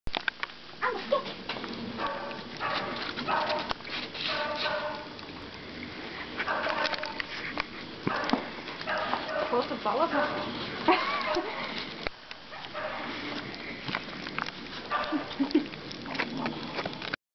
Sneeuwpret Amber
Amber speelt lekker in de sneeuw.